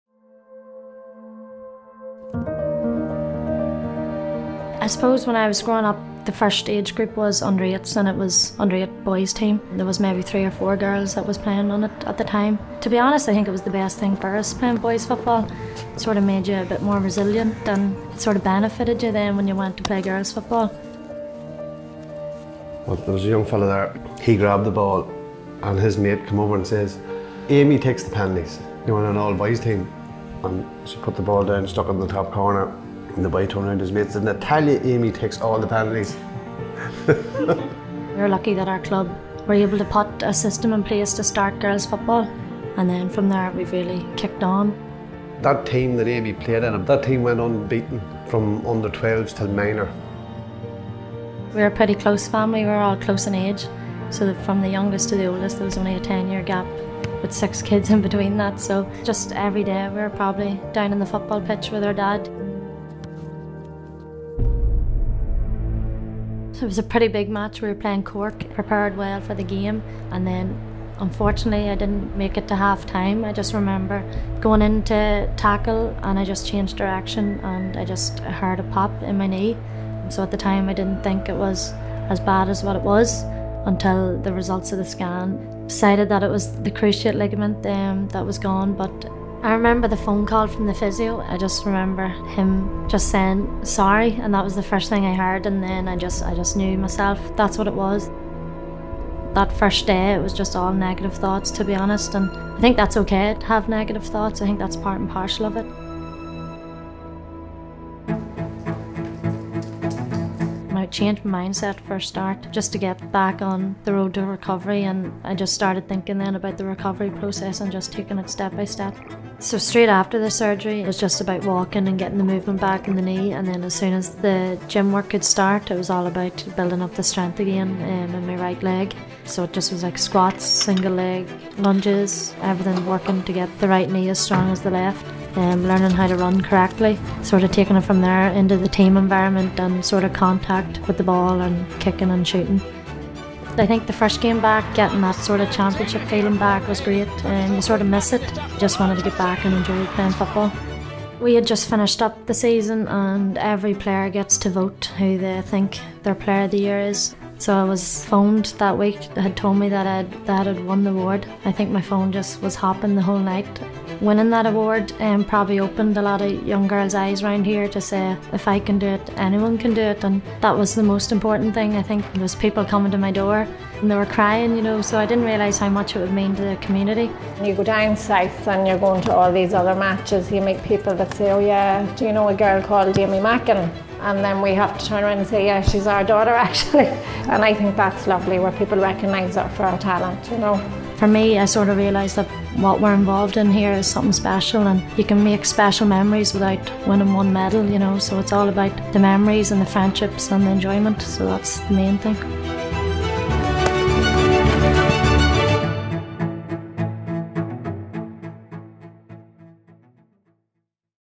"It's all about the memories" - AIG's One Tribe - Armagh's Aimee Mackin
“I realise that what we are involved in here is something special and you can make special memories without winning one medal. The main thing is that it’s all about the memories, the friendships and the enjoyment.”

AIG’s One Tribe Series showcases the effort players put in through the good times and the tough times.

This week we are delighted to hear from Armagh star and former Senior Player’s Player of the Year, Aimee Mackin. Aimee talks about playing Gaelic Football every day with her siblings during her childhood, her strength and determination to recover from her cruciate ligament knee injury, to finally being voted as the Senior Player’s Player of the Year in 2020.

AIG is the “Official Insurance Partner” to the LGFA.